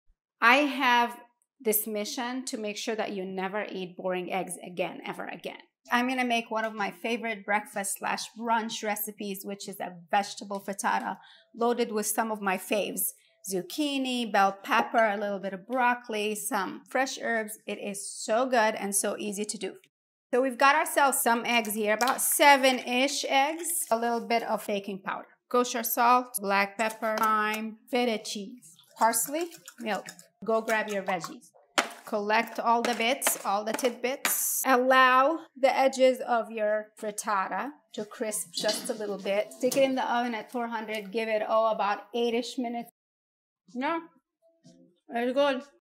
Brunch is served! Mediterranean Veggie Frittata!
This vegetarian Vegetable Frittata is perfectly balanced with earthy roasted vegetables, rich extra virgin olive oil, and sharp feta cheese!

INGREDIENTS:
🔹 Extra virgin olive oil (I used Early Harvest Greek EVOO)
🔹 1 small red bell pepper cored and chopped
🔹 1 small zucchini small diced
🔹 2 green onions white and light green parts only, roughly chopped
🔹 4 ounces broccoli cut into small florets
🔹 Kosher salt and black pepper
🔹 ¼ teaspoon baking powder optional
🔹 ¼ cup whole milk
🔹 ⅓ cup feta cheese crumbled, more to serve
🔹 ⅓ cup finely chopped fresh parsley more to serve
🔹 1 teaspoon fresh thyme